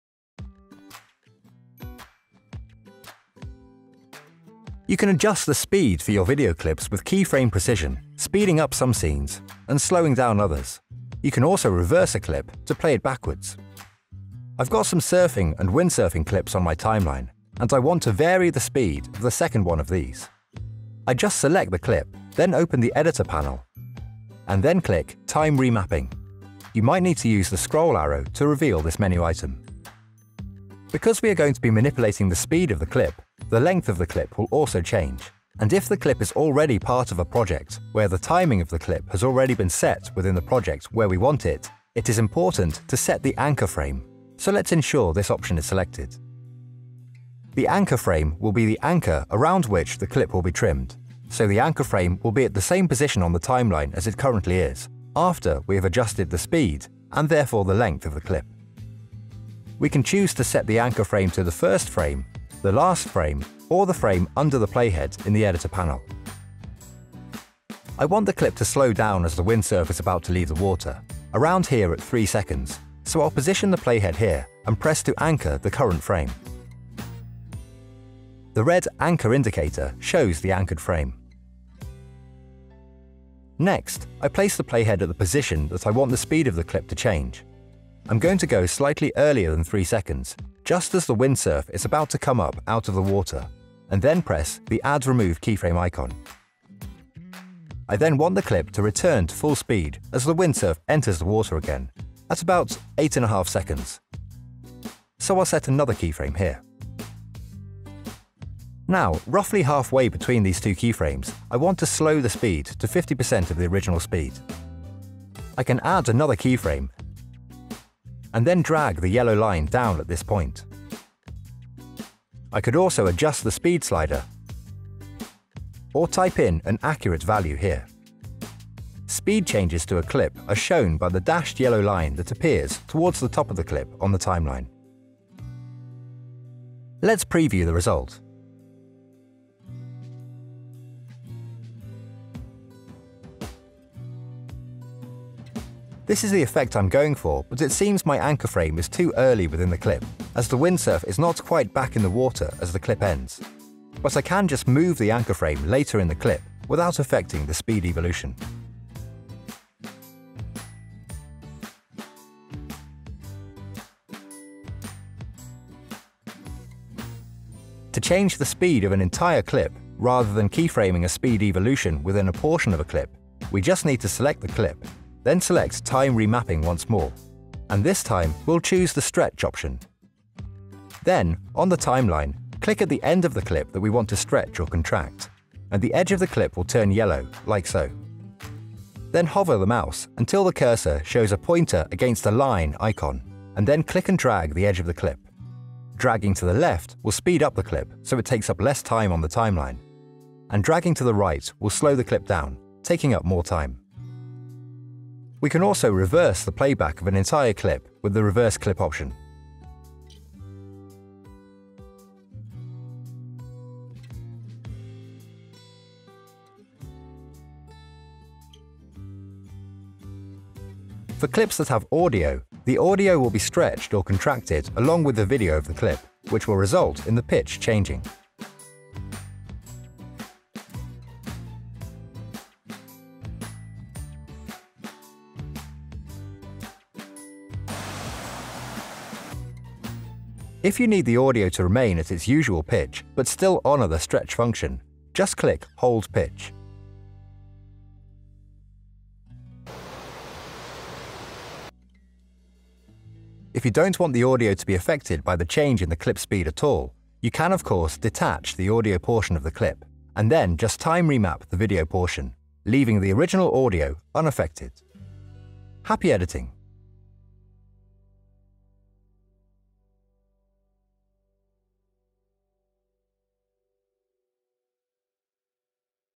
Time Remapping in Pinnacle Studio
Play with the video speed with Time Remapping and easily add slow motion or high speed effects to your videos. Even reverse and replay your scenes. Simply select your clip, go into the Editor, and select Time Remapping to set the speed to create slowmo or high-speed effects.